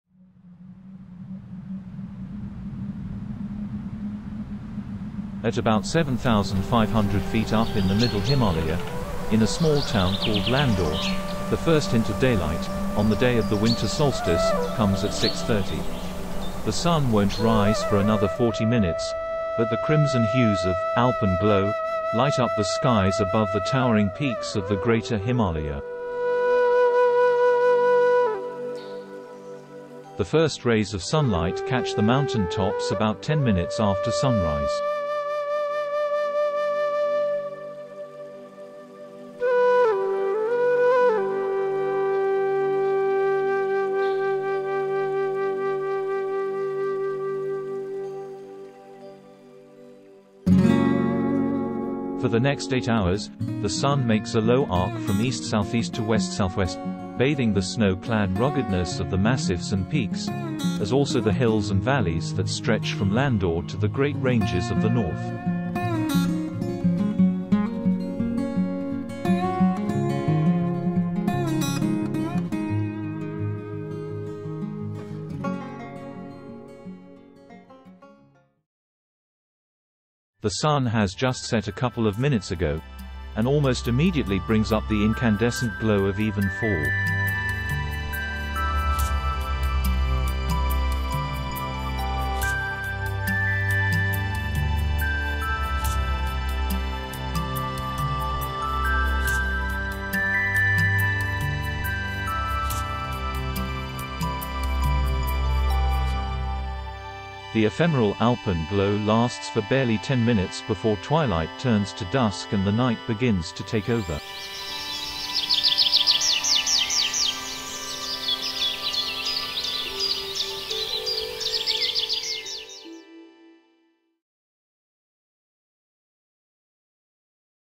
Garhwal Himalaya - View from Landour
Views of the himalayas from Landour -- daybreak to dusk